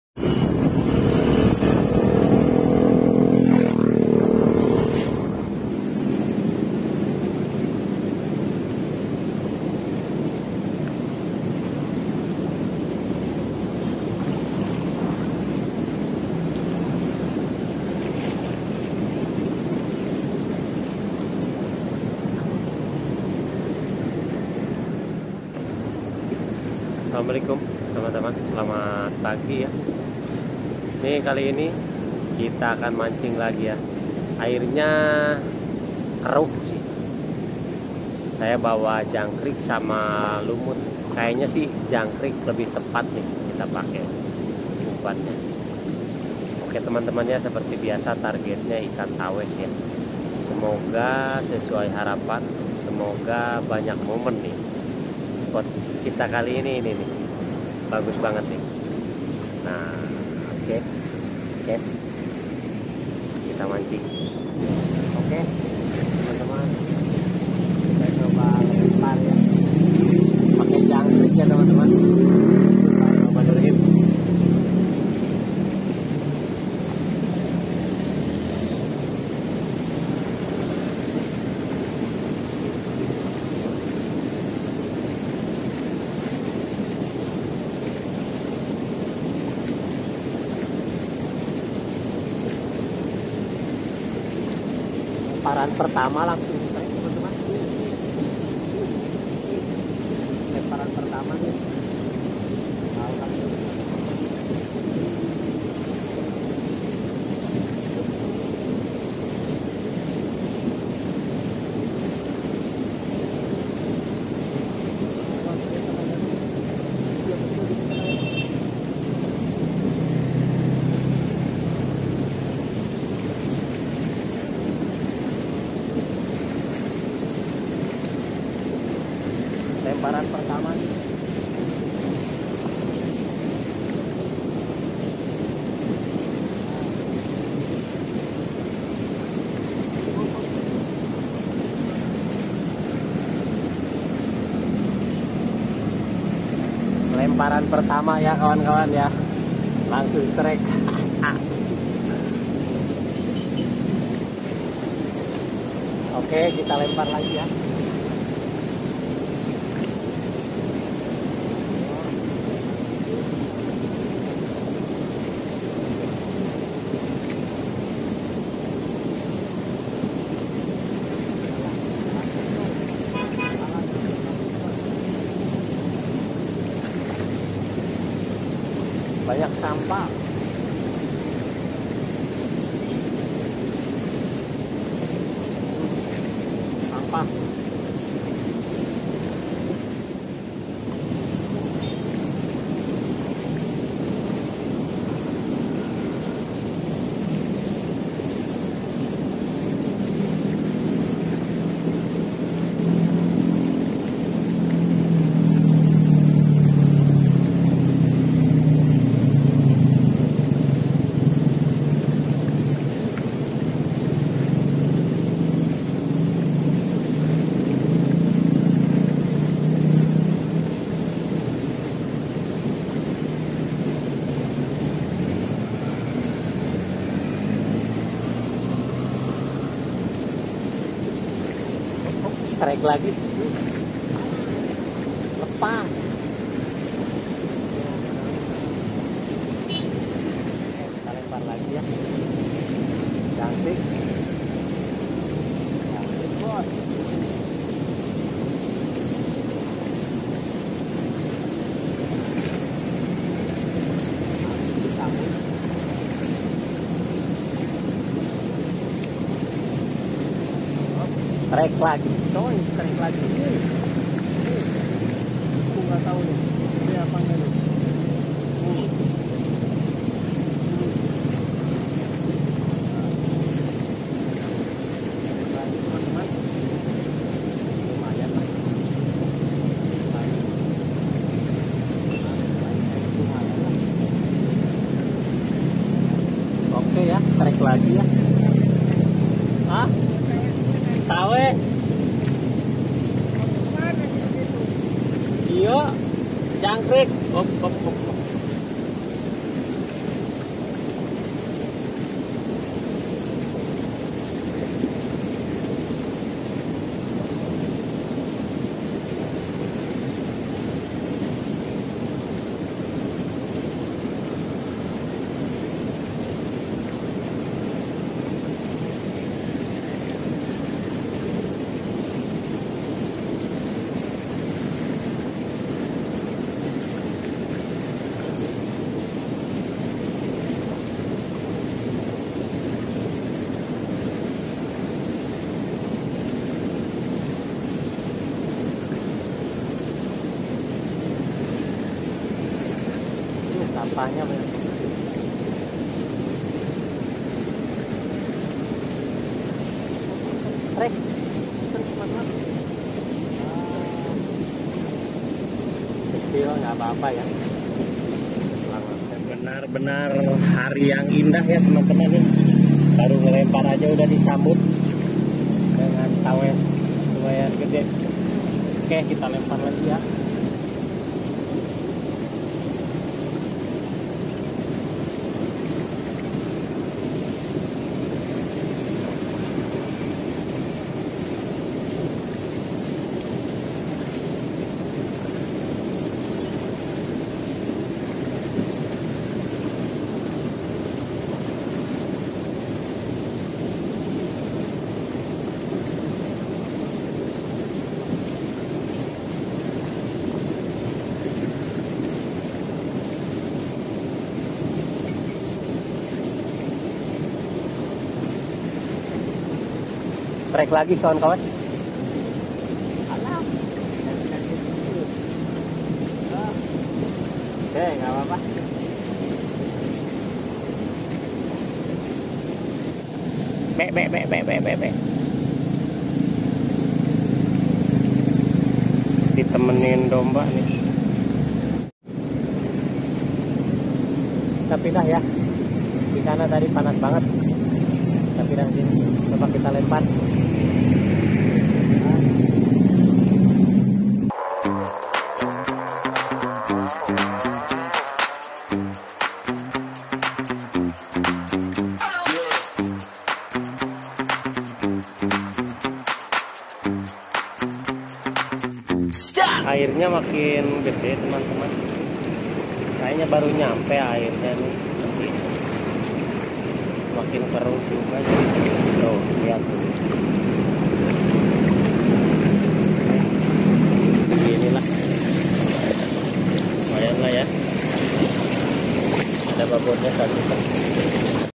gunakan umpan jangkrik saat air keruh
umpan jangkrik adalah salah satu makanan alami ikan tawes terlebih saat air keruh, ikan tawes sangat cocok di kasih umpan jangkrik
teknik mancing ikan tawes arus deras